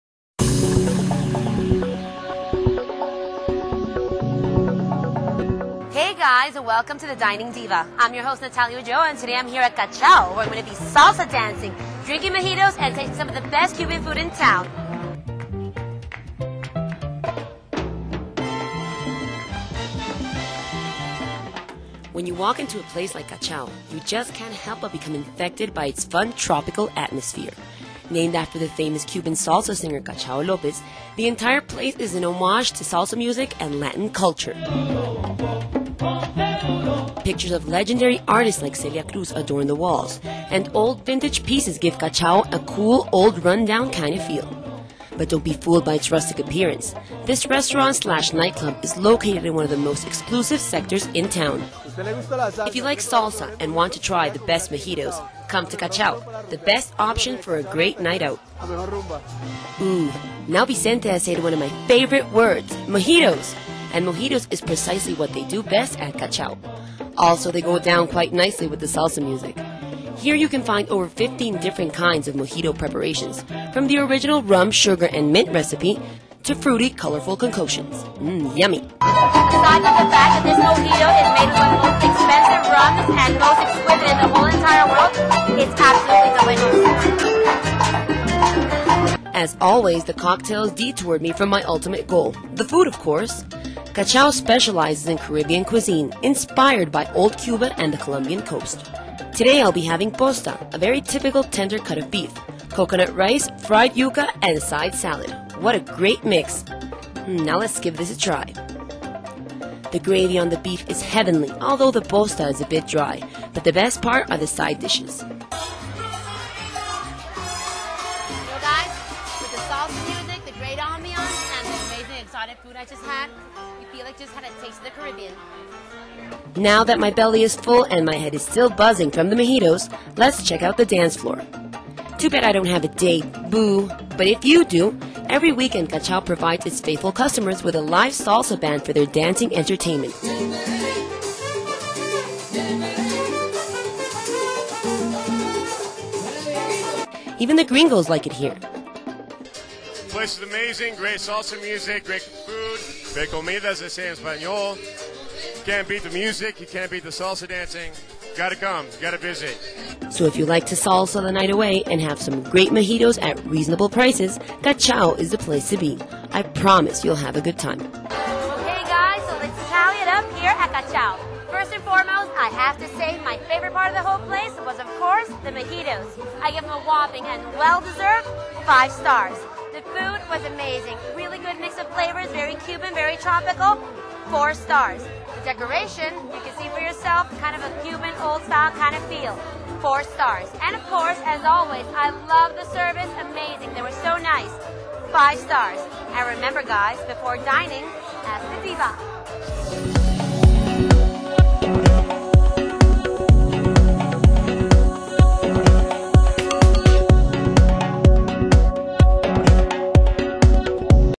Dining Diva at Cachao
Our Dining Diva visits Cachao. The restuarant is known for great mojitos, food, and salsa dancing.